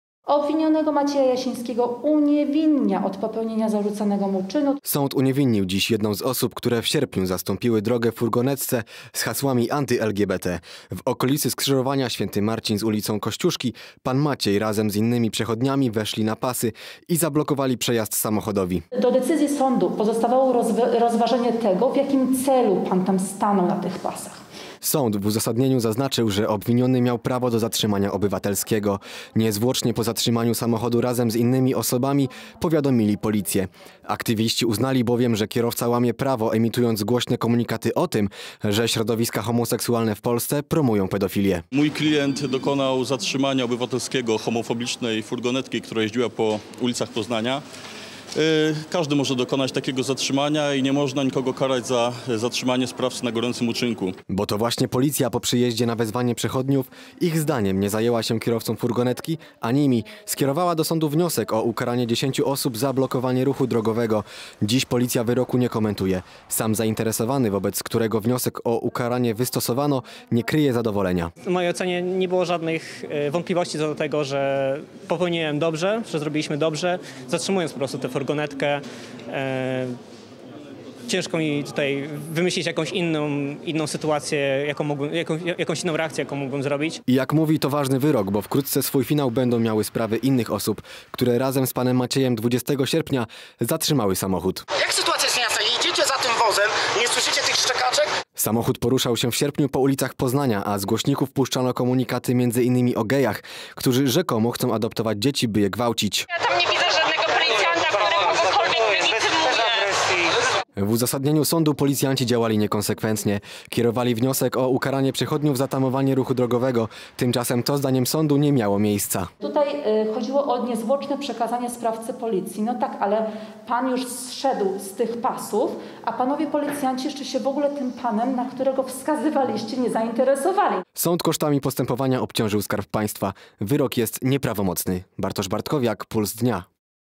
To było obywatelskie zatrzymanie. Tak zadecydował sąd
Sąd odmówił ukarania aktywisty, który wraz z innymi osobami pod koniec sierpnia zablokował w centrum Poznania furgonetkę z hasłami wymierzonymi w osoby LGBT. Samochód emitował komunikaty, w których padały stwierdzenia, że homoseksualiści promują pedofilię. Na ławie oskarżonych zasiadł jednak mężczyzna, który zdaniem policji tamował ruch. Jednak dziś sąd wskazał na niekonsekwencję policji.